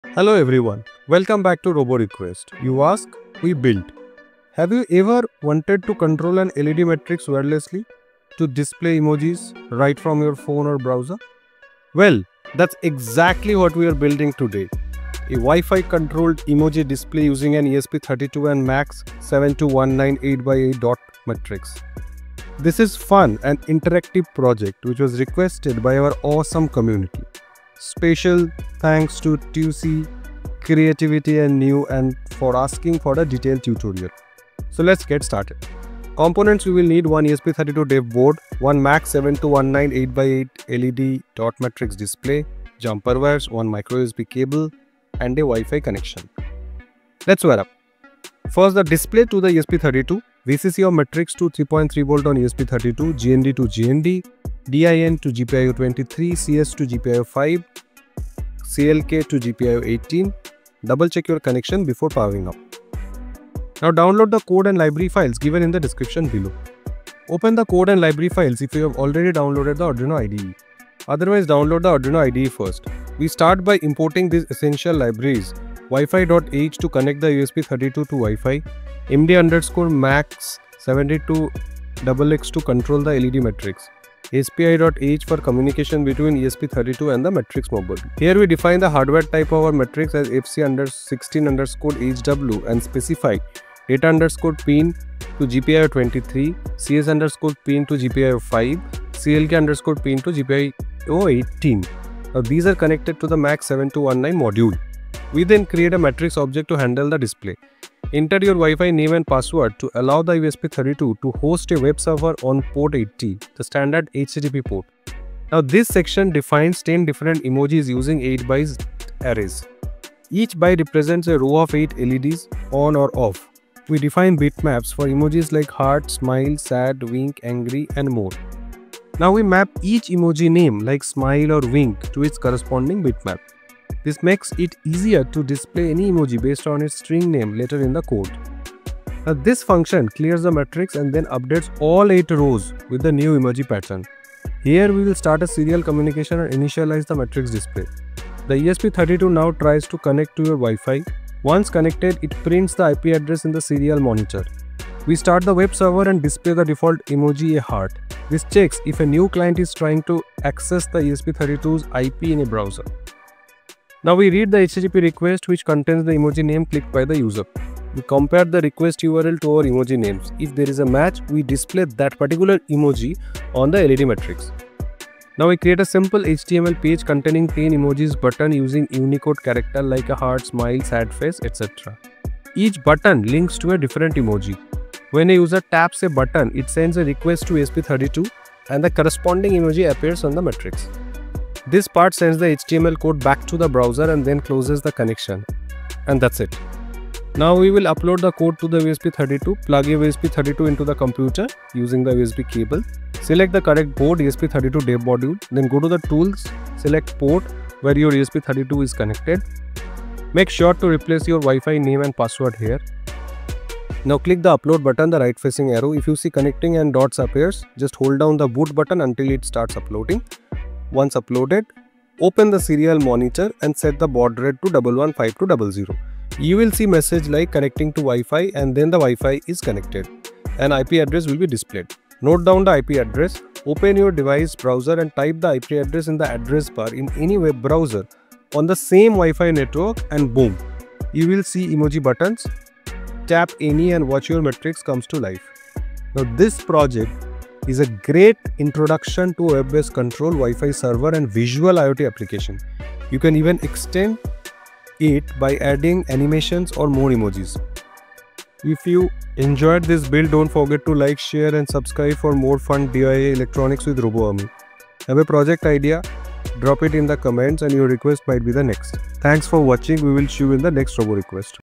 DIY Emoji Display Using ESP32 & LED Matrix | Change Faces via Wi-Fi!
Welcome to RoboRequest – You Ask, We Build!
This time, we’re adding emotions to electronics with an interactive Emoji Display! 😄❤️😢
Control an 8x8 LED Matrix using your phone or computer via Wi-Fi, powered by an ESP32. Just tap an emoji on the web browser and watch it light up on the matrix — wirelessly!

📲 No mobile app, no extra controller — just Wi-Fi, emojis, and the magic of code.

💡 This is a perfect project for learning ESP32 web server basics, creating interactive interfaces, and working with LED matrix graphics — all while having emoji-filled fun!

📍 What You’ll Learn in This Tutorial:
✅ How to interface an 8x8 LED Matrix with ESP32
✅ Create a web server using ESP32
✅ Use buttons in HTML to trigger matrix display changes
✅ Display 10 different emojis on the LED grid
✅ Write efficient code to map emojis to bitmaps

🧰 Components Used:
ESP32 Dev Board
8x8 LED Matrix (MAX7219 module)
Jumper Wires
Breadboard
USB Cable

🕹 Chapters (Timestamps):
0:00 – Intro
0:18 – Project Overview
0:45 – Components Required
1:10 – Circuit & Connections
1:35 – Web Server Interface Explained
2:15 – Code Walkthrough
4:00 – Live Demo: Emoji on Matrix
4:45 – Final Thoughts

📌 Recommended For:
IoT + WiFi Projects
LED Matrix Animations
Creative Web Control Projects
Beginner ESP32 Learners
STEM + Classroom Demos
Wi-Fi Based Interactions

📢 Special Thanks to:
@tsui-fengwu4344 and @CreativityandnNew for requesting this awesome project idea! 🙌

💬 Drop your RoboRequest in the comments — we might build your idea next!